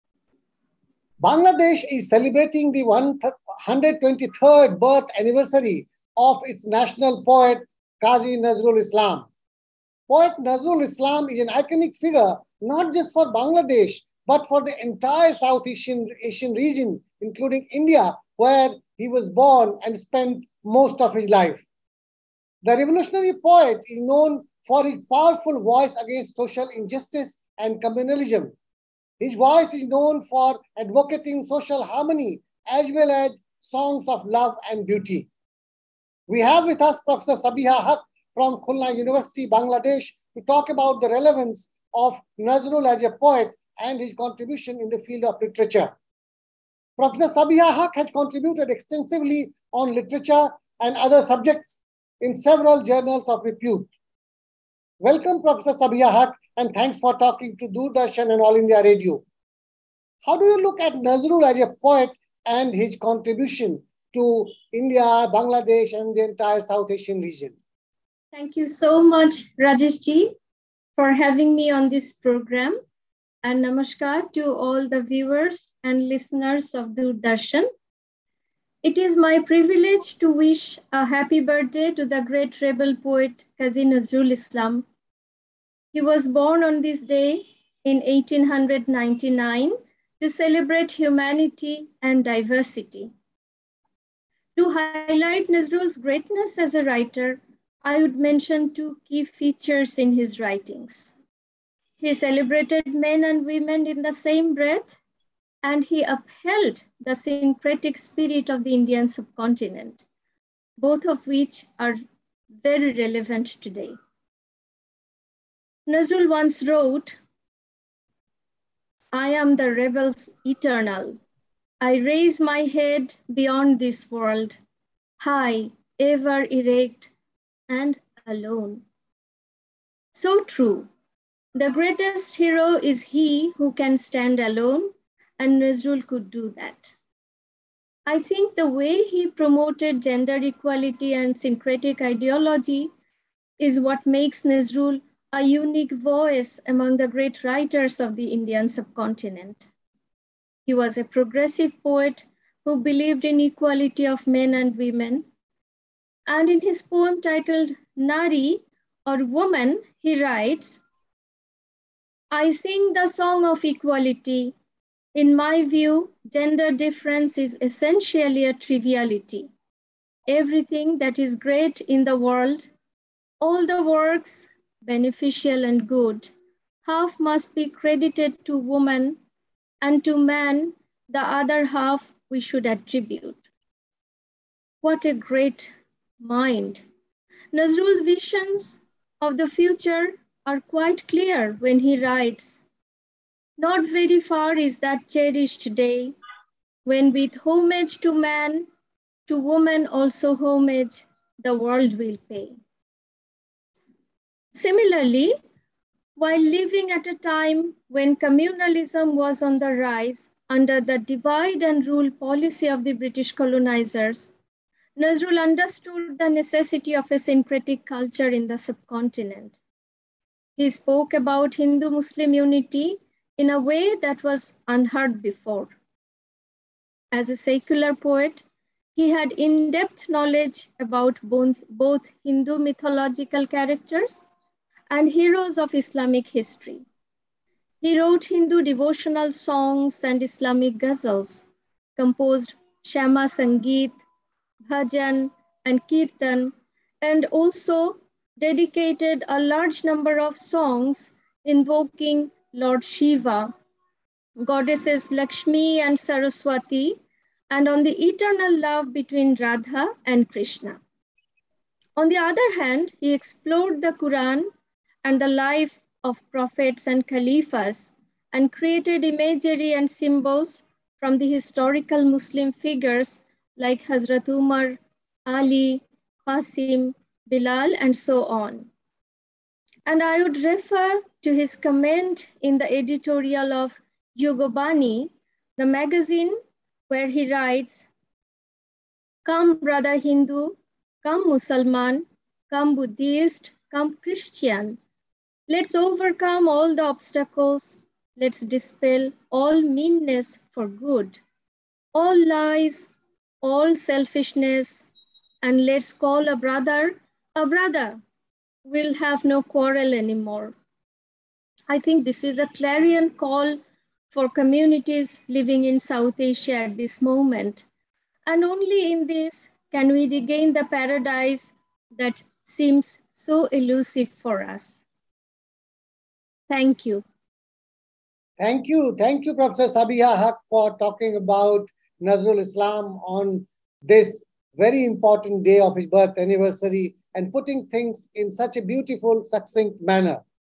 Interview with Prof. Sabiha Huq on the occasion of the birth anniversary of poet Kazi Nazrul Islam
Bangladesh is celebrating the 123rd birth anniversary of its national poet Kazi Nazrul Islam. Poet Nazrul is an iconic figure not just for Bangladesh but for the entire south Asian region including India where he was born and spent most of his life.
Prof. Sabiha Huq of Khulna University talks about the contribution of poet Nazrul and his views about the world.
Prof. Sabiha Huq Professor of English at Khulna University. She takes interest in Theatre, translation, postcolonial literature and women’s writings.  Her latest publication is "The Mughal Aviary: Women’s Writings in Pre-modern India". She is the editor of "Dead Metaphor", a bilingual literary magazine.